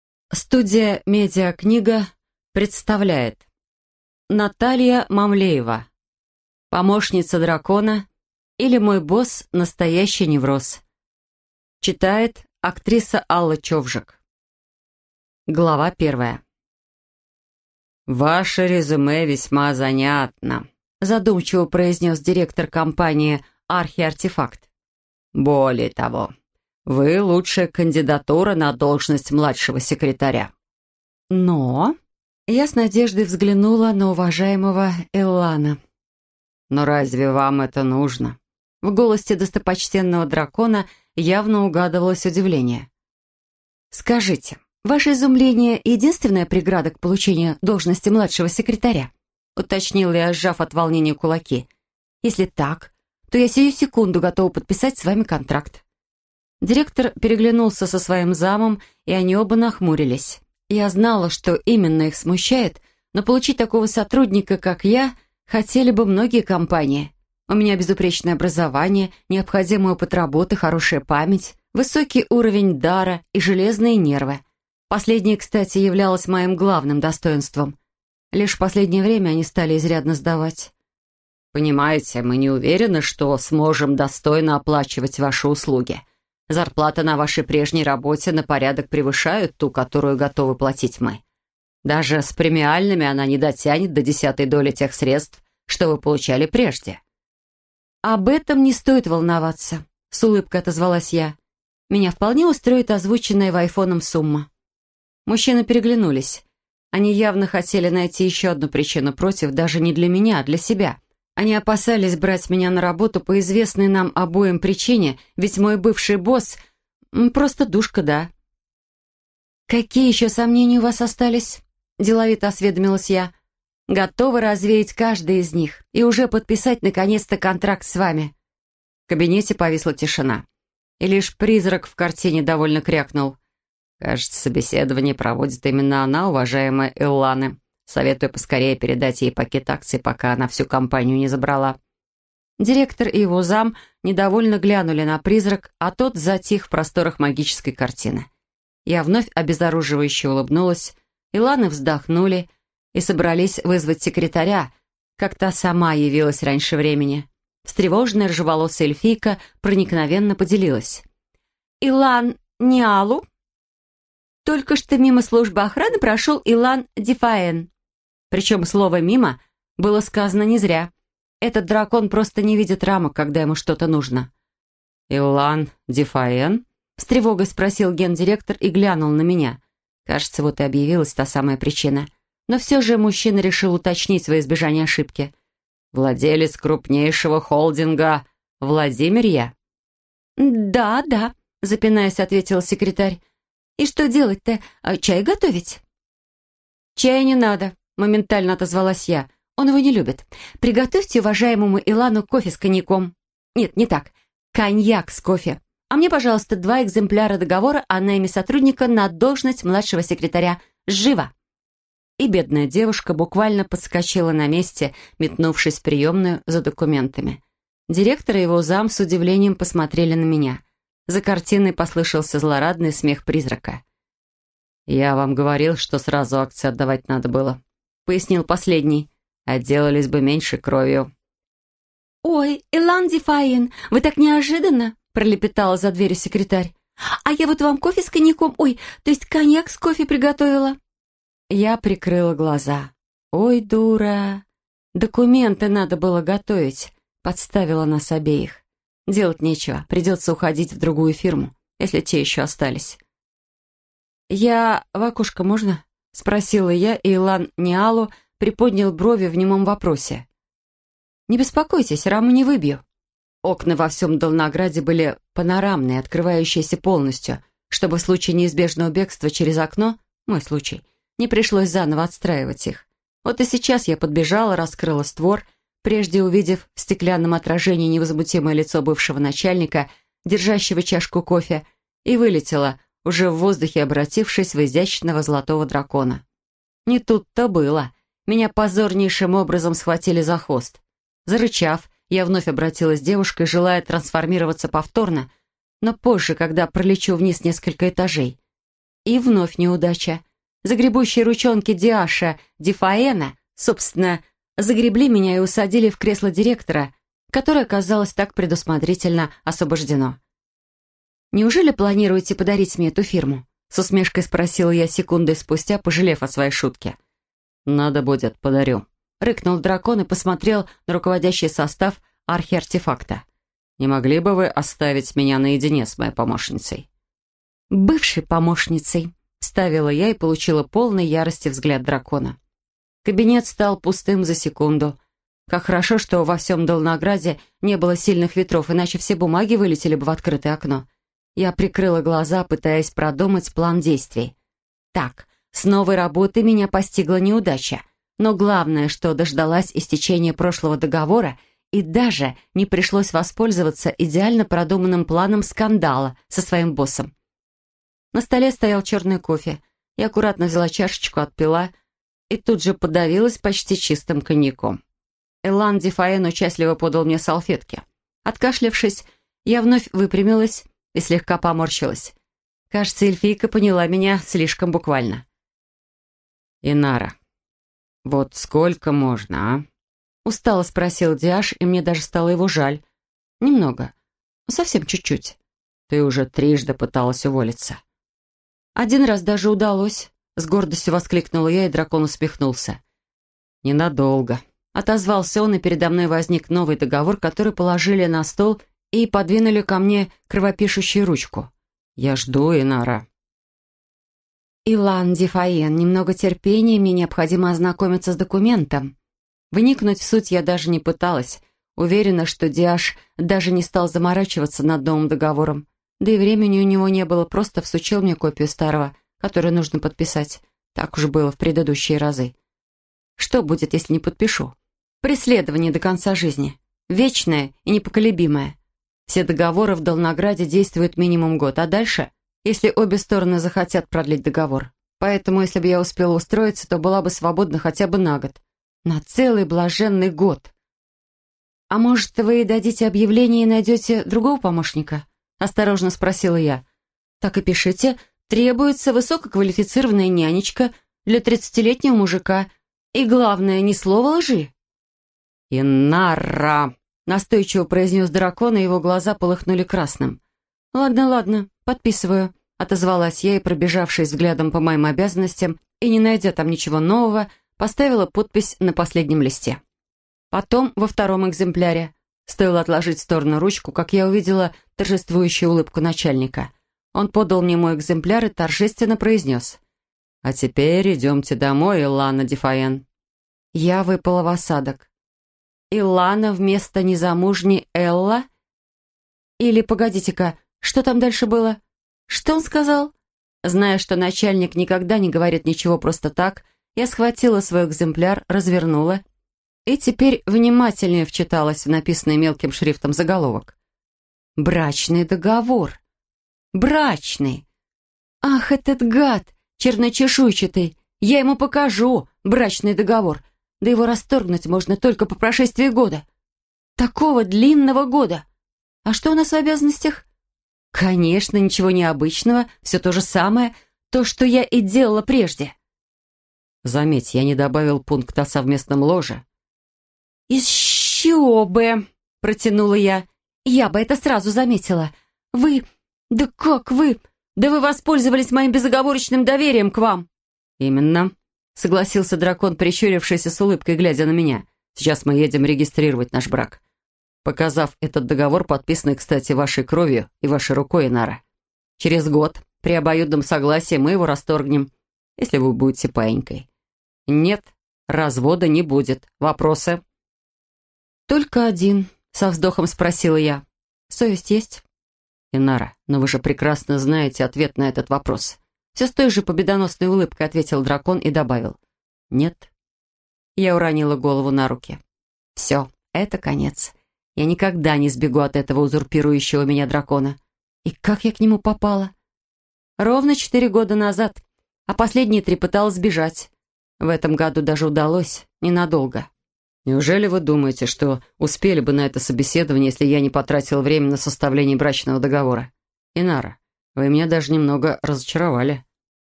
Наталья Мамлеева – Мой босс – настоящий невроз, или Помощница дракона. [Аудиокнига]
Аудиокнига Мой босс – настоящий невроз, или Помощница дракона Натальи Мамлеевой – слушать онлайн.
Автор: Наталья Мамлеева.
Жанры: любовное фэнтези, юмористическое фэнтези
Дата выхода на ЛитРес: 21 апреля 2020
Дата написания: 2020
Чтец: Алла Човжик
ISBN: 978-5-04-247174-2

Описание: Студия «МедиаКнига» представляет аудиокнигу популярной современной писательницы Натальи Мамлеевой – «Мой босс – настоящий невроз, или Помощница дракона».  Книга прочитана популярной актрисой и радиоведущей Аллой Човжик. Как сильно нужно желать увольнения, чтобы выйти замуж за собственного босса? Могу с уверенностью заявить – сильно, очень, но не настолько, чтобы идти на такие крайние меры! Но кто бы меня спрашивал?.. Теперь приходится терпеть свекровь, помешанную на идее продления рода черных драконов, и совмещать семейную жизнь с работой. Но вот проблема – кто-то догадывается о секрете моего начальника, а я начинаю Слышать зов могущественного артефакта…. Слушаем, лайкаем, активно комментируем!